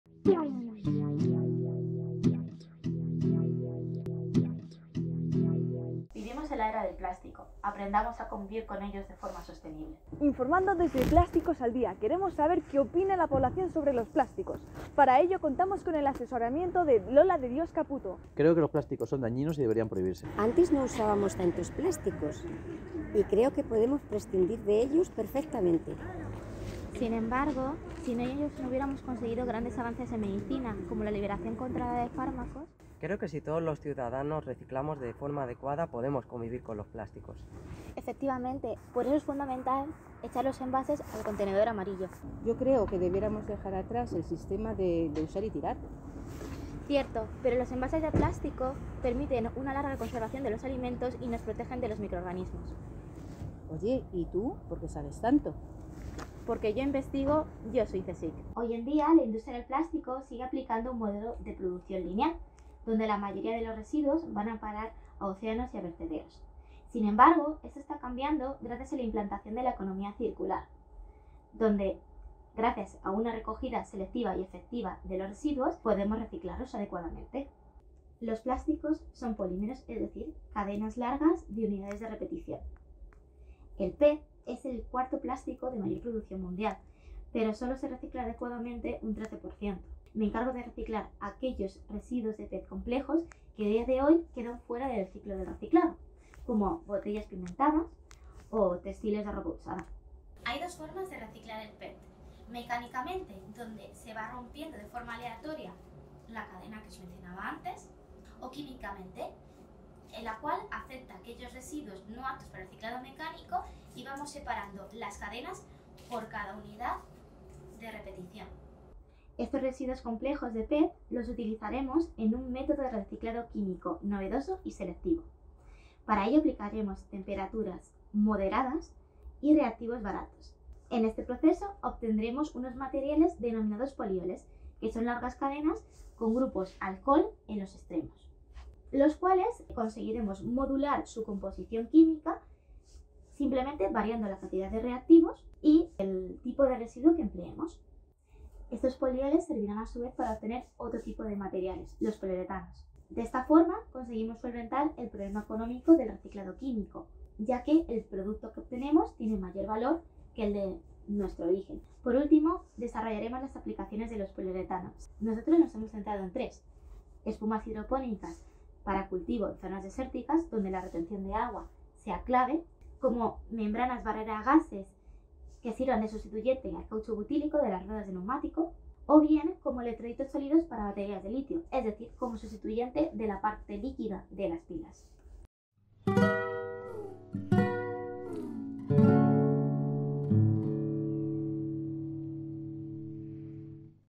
V Edición Yo investigo. Yo soy CSIC: María Dolores de Dios Caputto - Reciclado Químico.
Reciclado Químico Sostenible, del PET al poliuretano A través del reportaje ficticio “Plásticos al Día”, la estudiante de doctorado Mª Dolores de Dios Caputto presenta la opinión generalizada que tiene la población sobre los plásticos. Para ello, se eligen distintos escenarios donde los plásticos juegan un papel fundamental en nuestra vida cotidiana, y se muestra su importancia con datos constatados. Mª Dolores explica la importancia del reciclaje según el modelo de la Economía Circular, tomándolo como referencia en su tesis. Con ello, desarrolla un método de reciclado químico novedoso y selectivo, donde se introducen por primera vez residuos complejos de PET que actualmente quedan fuera del ciclo de reciclado. Estos residuos servirán para obtener un material de mayor valor económico, el poliuretano, haciendo que este proceso sea asequible para la industria. Sustainable Chemical Recycling, from PET to polyurethane Through the fictional report "Plastics a Day", PhD student Mª Dolores de Dios Caputto presents the general opinion of the population on plastics. For this purpose, different scenarios are chosen in which plastics play a fundamental role in our daily lives, and their importance is shown with contrasted data. Mª Dolores explains the importance of recycling according to the Circular Economy model, taking it as a reference in her thesis. To this end, she develops a novel method of selective chemical recycling, in which complex PET waste that is currently left out of the recycling cycle is introduced for the first time. This waste will be used to obtain a material of higher economic value, polyurethane, making this process affordable for industry.

Plásticos, Reciclado, Economía Circular, Residuos, PET